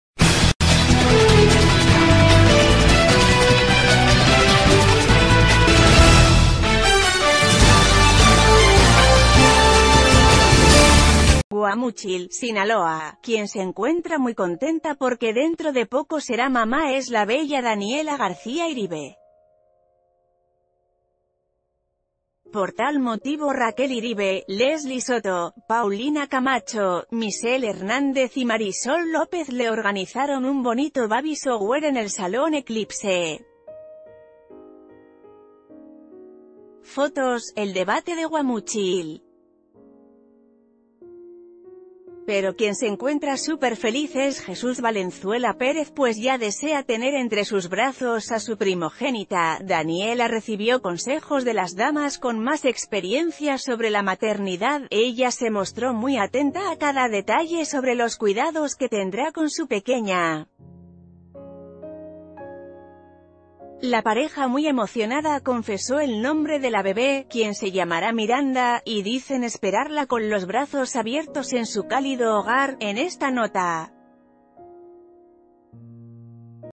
En espera de su princesa
Guamúchil, Sinaloa.- Quien se encuentra muy contenta porque dentro de poco será mamá es la bella Daniela García Iribe. Por tal motivo Raquel Iribe, Leslie Soto, Paulina Camacho, Michelle Hernández y Marisol López le organizaron un bonito baby shower en el salón Eclipse. Fotos: EL DEBATE de Guamúchil...